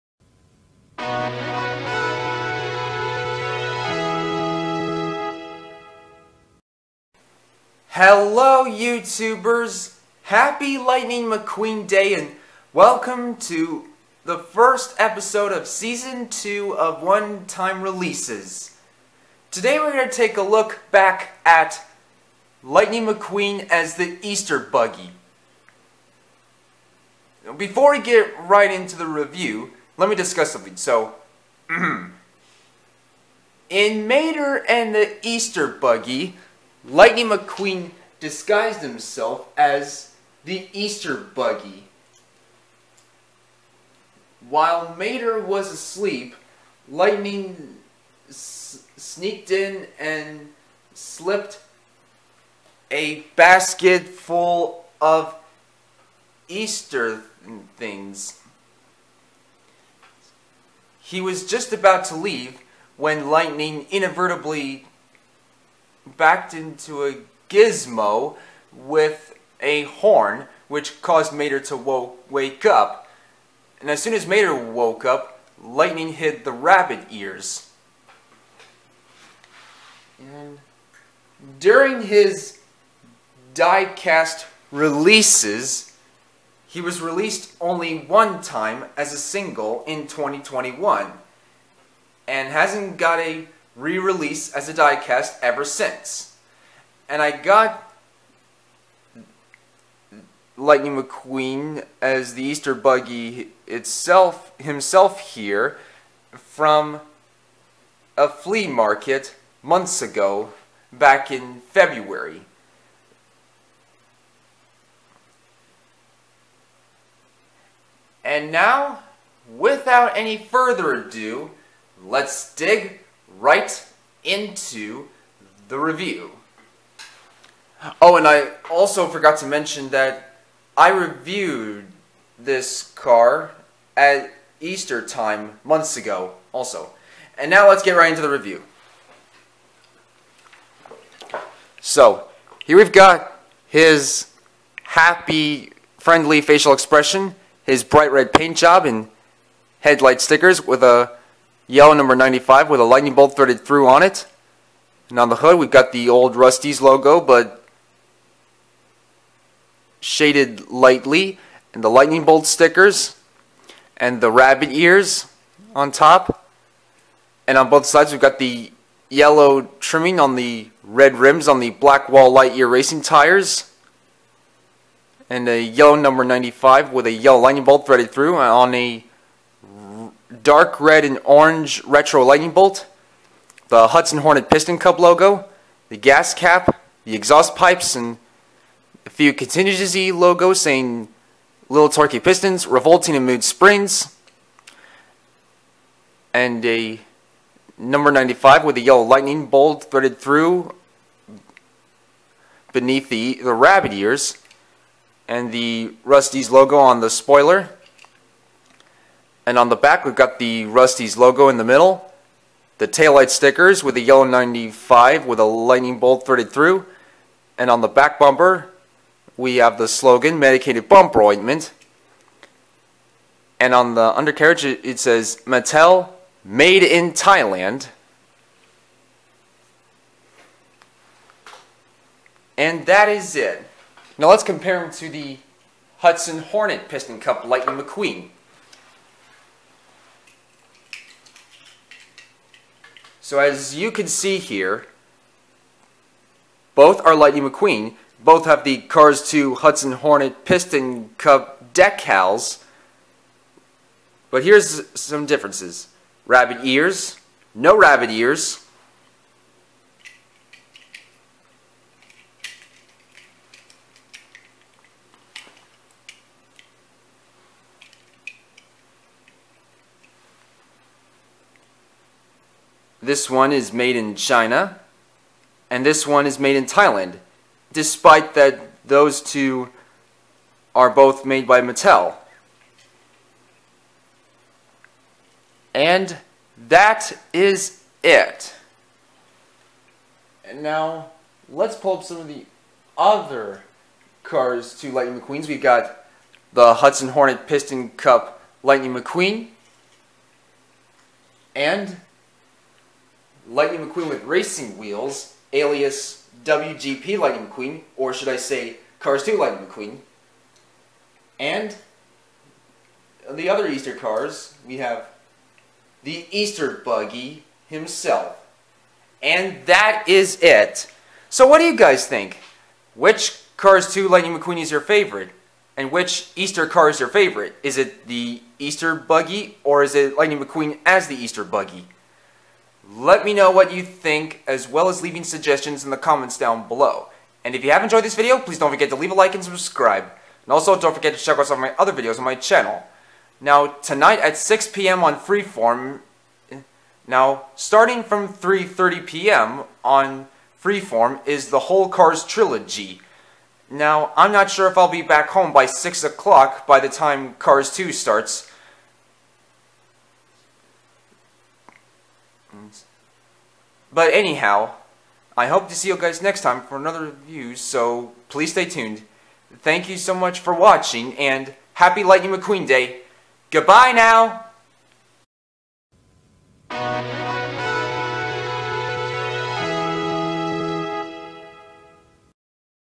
One time releases season 2 episode 1 Lightning McQueen as The Easter Buggy diecast
Hello everybody, Happy Lightning McQueen day & today, we're gonna take a look back at Lightning McQueen as The Easter Buggy from the Cars book Mater & The Easter Buggy. Today at 3:30PM is the whole Disney Pixar Cars trilogy on Freeform (Spectrum Channel #47). Let me know what you think as well as leaving suggestions in the comments section below!
0:00 Intro
0:33 Retrospective
2:18 Review
5:21 Closure
Cars belongs to Disney/Pixar.
Beetle/bug 4x4/Baja bug is a trademark of Volkswagen.